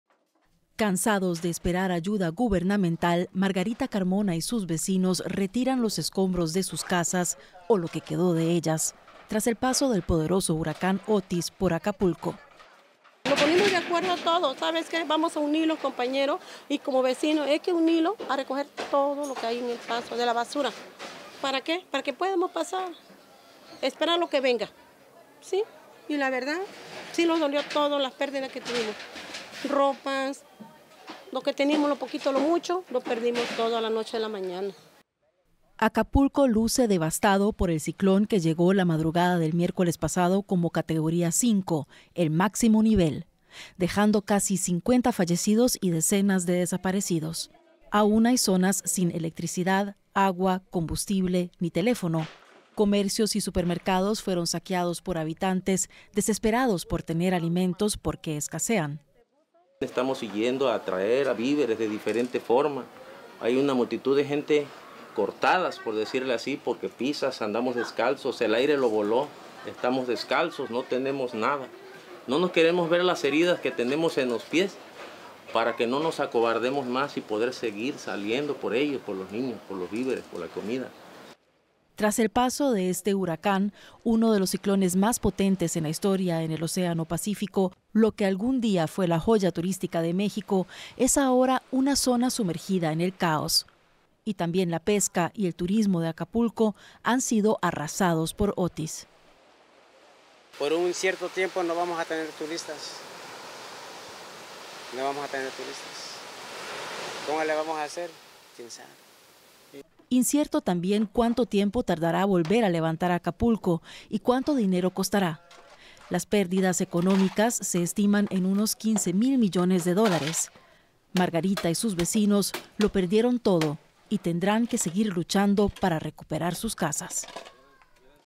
Acapulco recoge los restos que dejó el huracán Otis
La devastación ocasionada por la tormenta que azotó al balneario mexicano como huracán de categoría 5 deja a miles sin sus trabajos, negocios y hogares, mientras crece el descontento por la gestión de la crisis por parte del Gobierno de Andrés Manuel López Obrador.